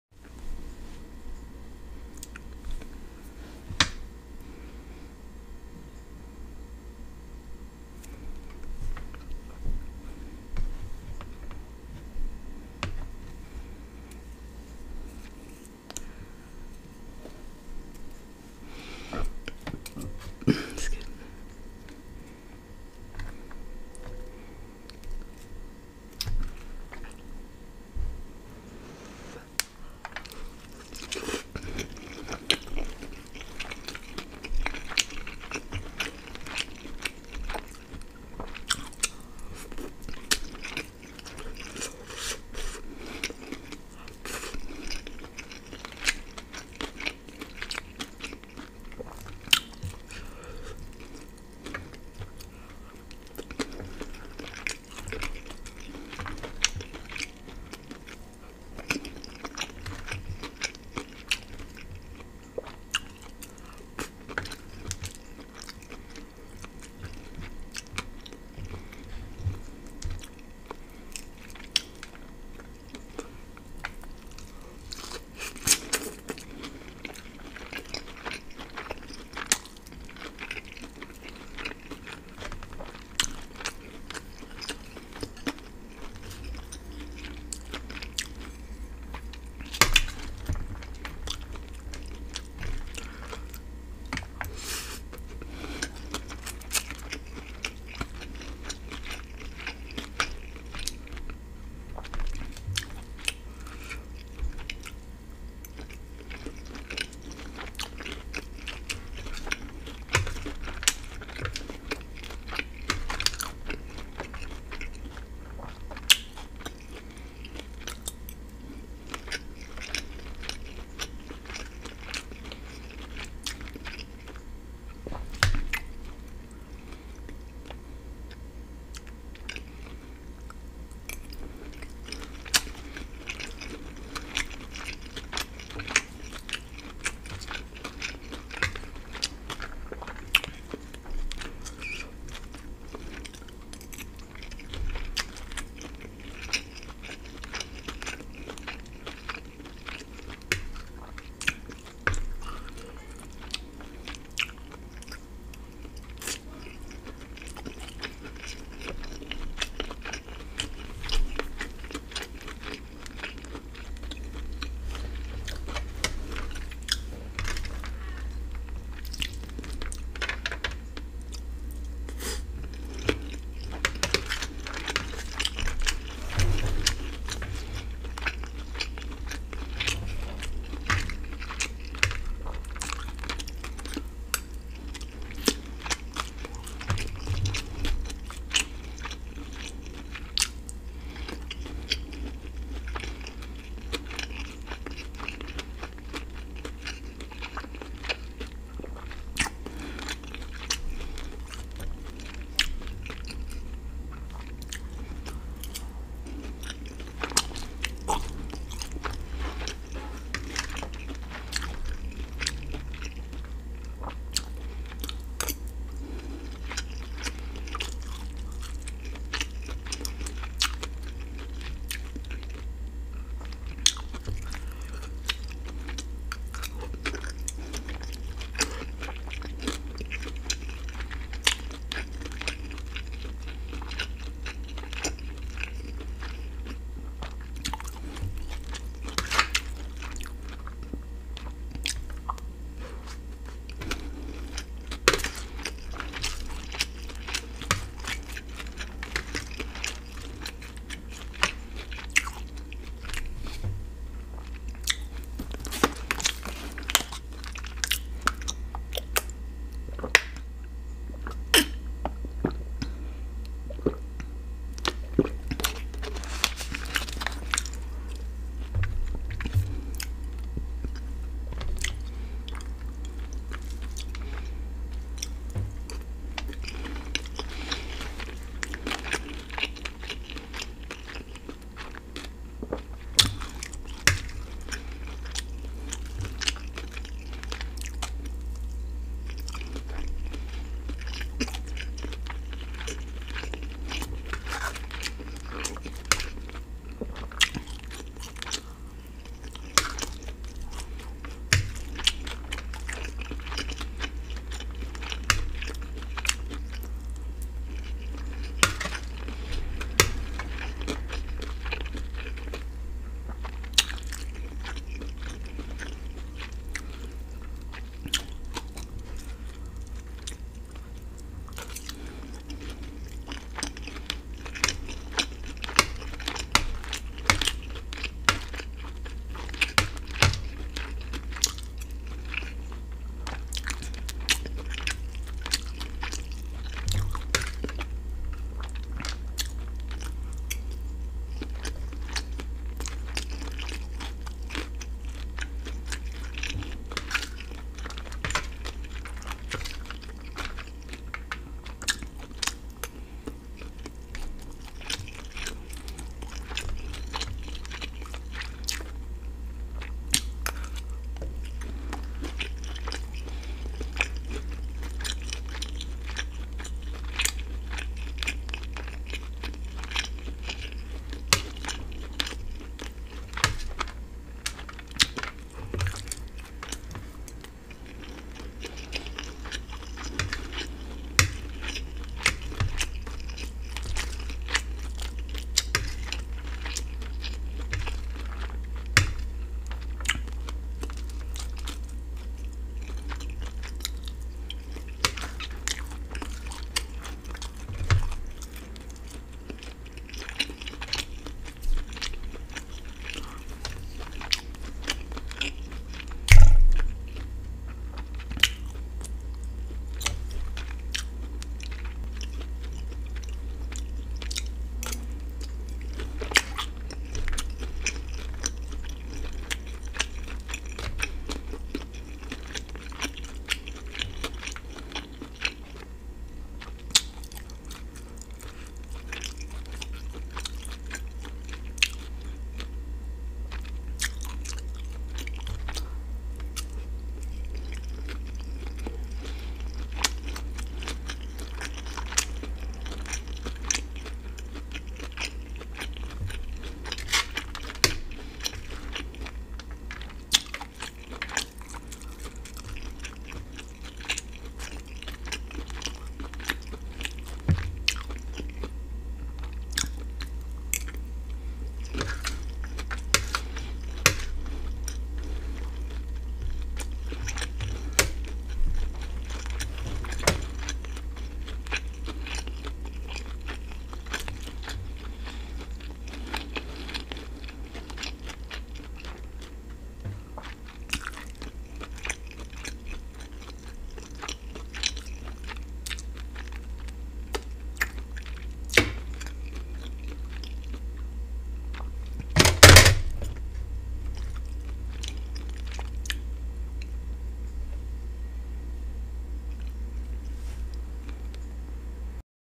ASMR Orange Chicken and Fried Rice ***Yummy***
Hello Friends for dinner I had some delicious Orange Chicken and Fried Rice I definitely wanted me a Shrimp Egg Roll but Ill wait til next time 😂😂😂😂..Let's Eat!